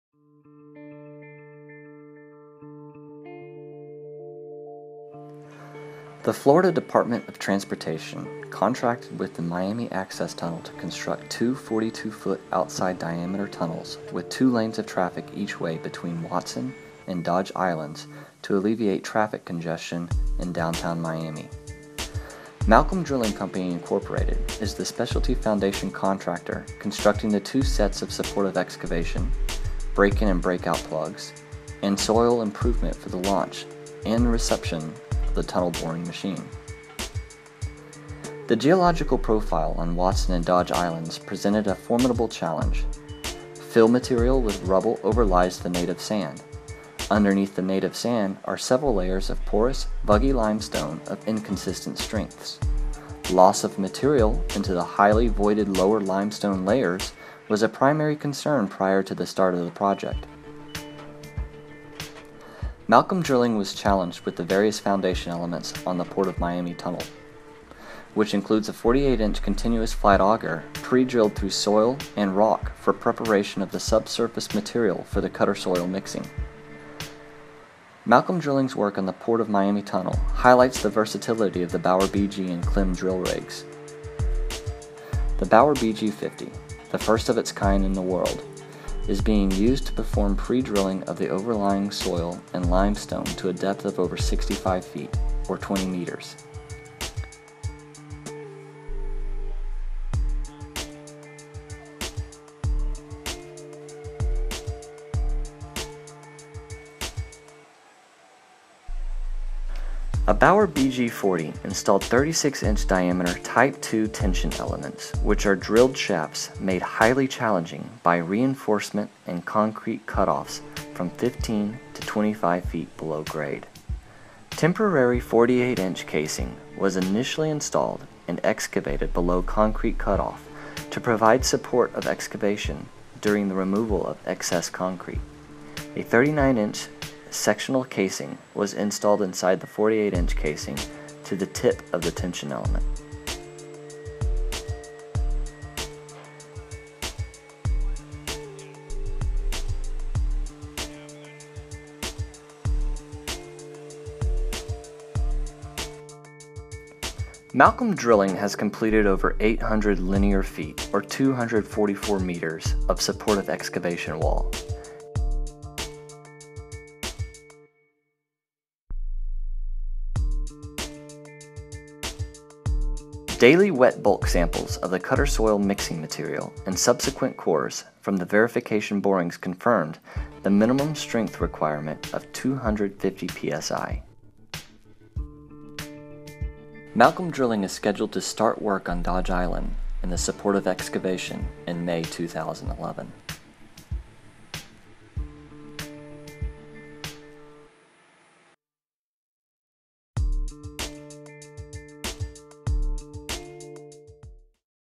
Port of Miami Tunnel Jobsite Story
Malcolm Drilling Company Inc. is the specialty foundation contractor for the port of Miami Tunnel. They are utilizing a BAUER BG 50, two BAUER BG 40s and a Klemm drill rig to create support of excavation, break in and break out plugs for the tunnel boring machine.